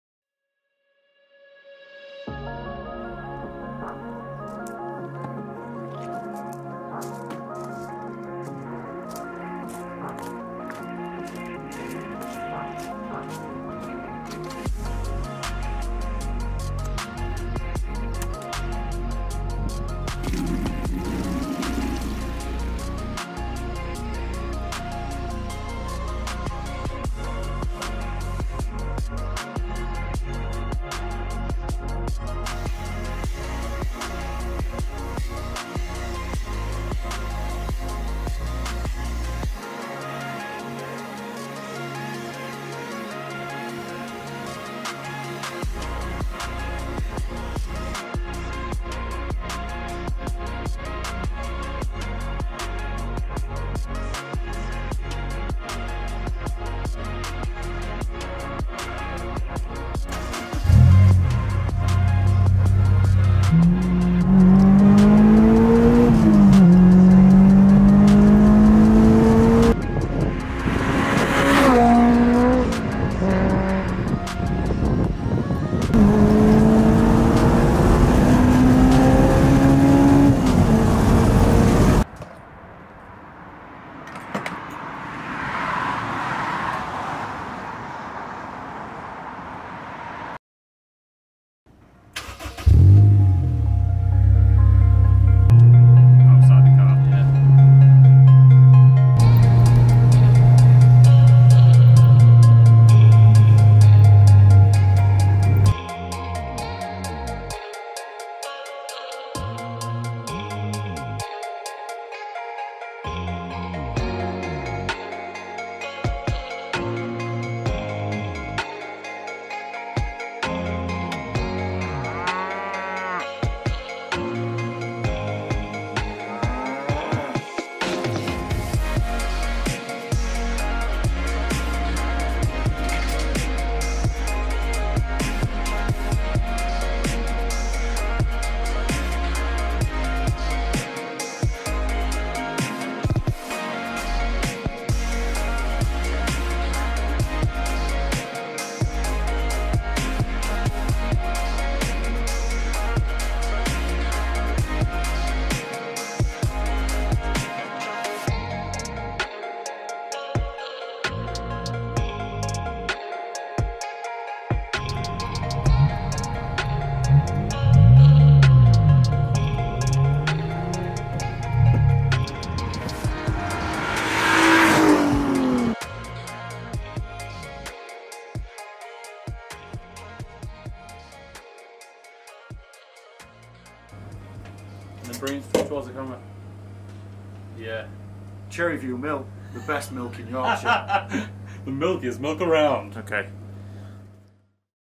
370z & S2000 Milk Run - The Drivers Den - 4K
What better to do on a Sunday than clean the Honda S2000, meet up everyone's favourite yellow Nissan 370Z and hit up some B roads?

If you like this style of video please let us know!

Huge thanks to Matt for helping out with the camerawork!

Track 1
Music released by Carbon Citizens